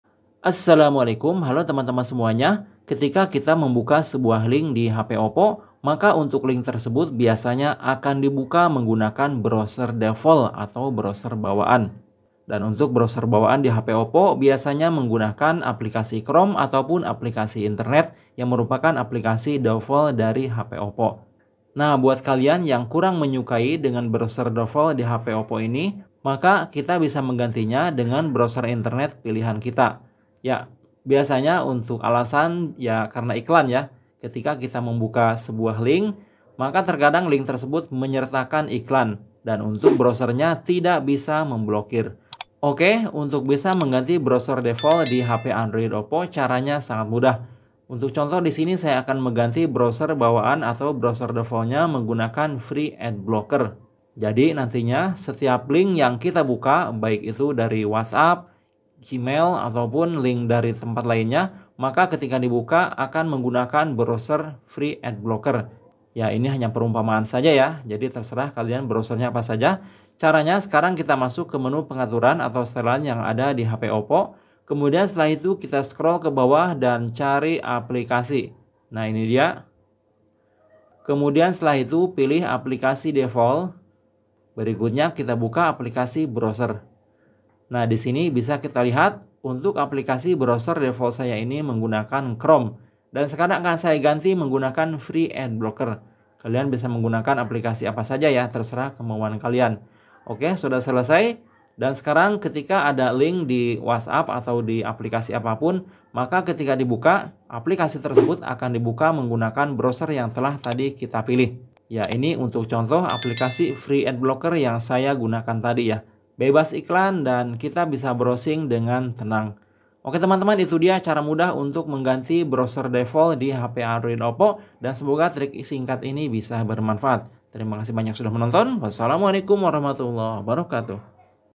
Cara Merubah Browser Default Di HP Oppo
Hai semuanya,ketika kita membuka sebuah link di Hp Oppo,maka untuk link tersebut biasanya akan dibuka menggunakan browser default atau browser bawaan,dan untuk browser bawaan di Hp Oppo biasanya menggunakan aplikasi Chrome ataupun aplikasi internet yang merupakan aplikasi default dari Hp Oppo.
Nah buat kalian yang kurang menyukai dengan browser default di HP Oppo ini maka kita bisa menggantinya dengan browser Internet pilihan kita.

Terimakasih